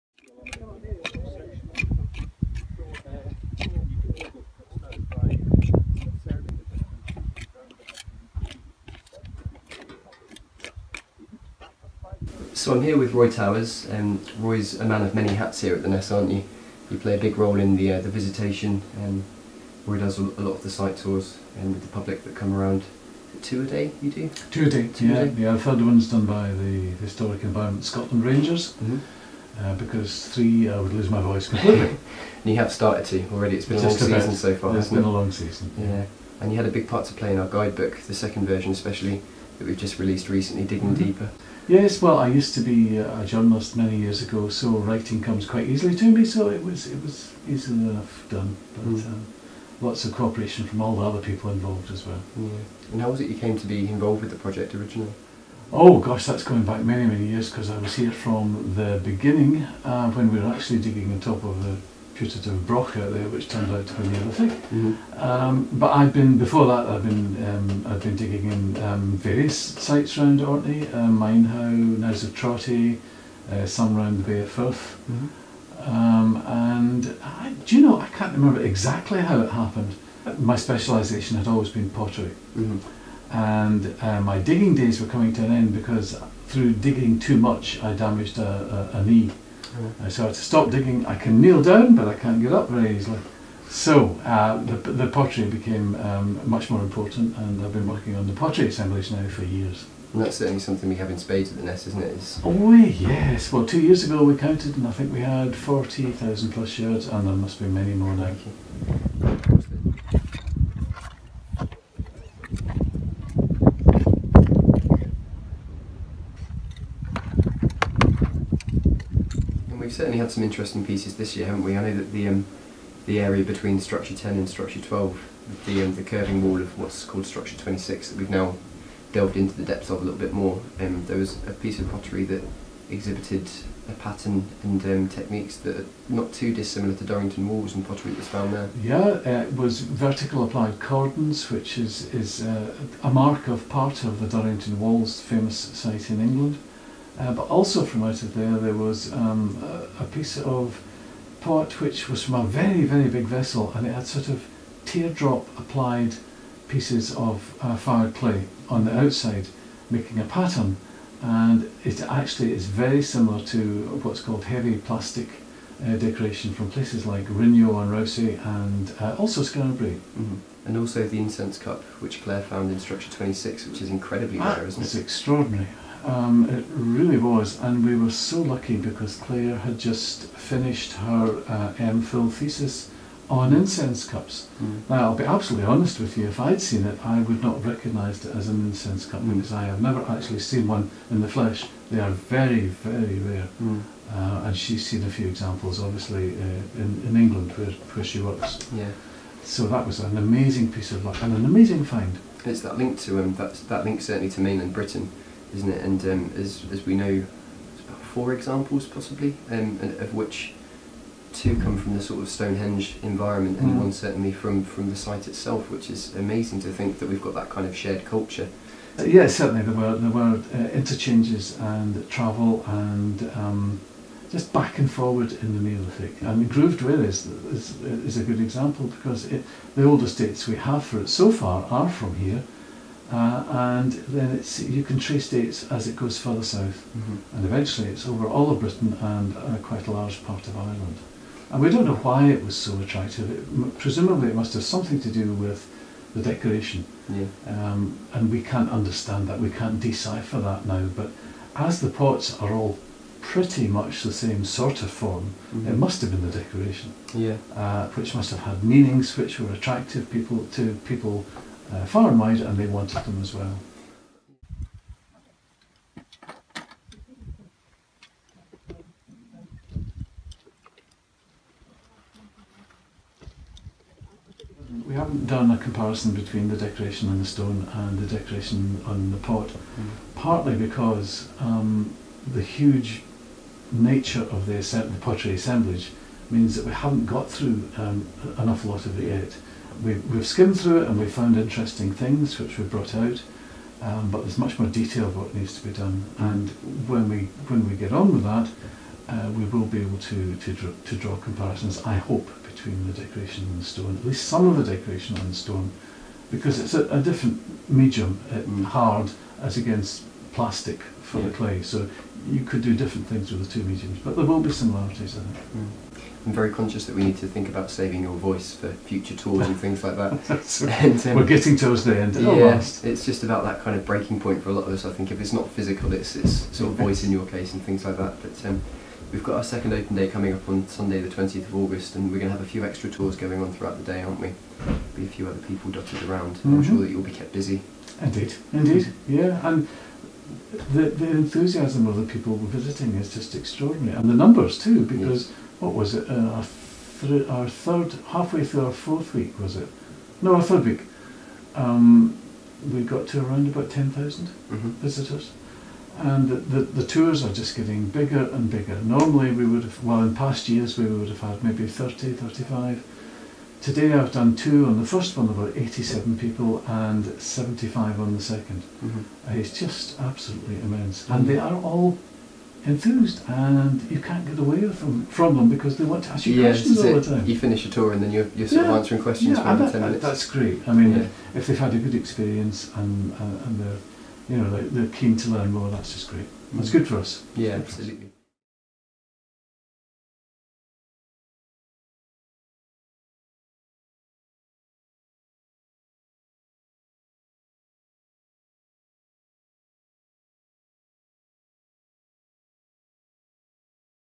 People, Place & Perception - Conversation with Roy Towers
Amongst the amazing discoveries unearthed at the Ness of Brodgar, are literally thousands of pottery fragments.

Roy Towers, pottery specialist at the University of the Highlands and Islands Archaeology Institute talks to Simon Gray (MSc Archaeology student at UHI Archaeology Institute on professional placement...if you didn't know already!) about the pottery, stone decoration and the social links developed in the Neolithic. for more info